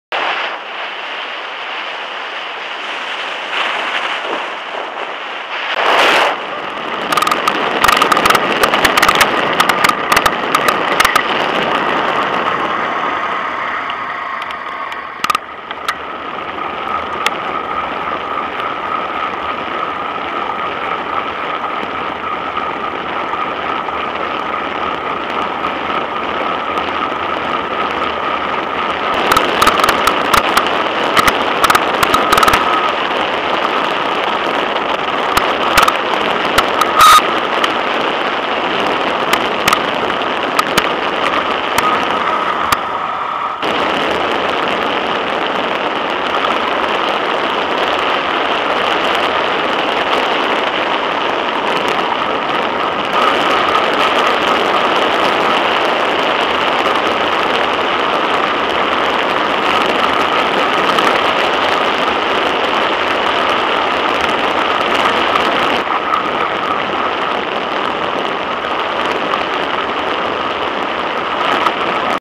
Montage Descente VTT
Petit montage vidéo de différents virages pris en descente avec mon VTT et notamment, même virage pris à la même allure filmé sous 2 vues différentes au début de la vidéo, avec une drift HD170 en 1080.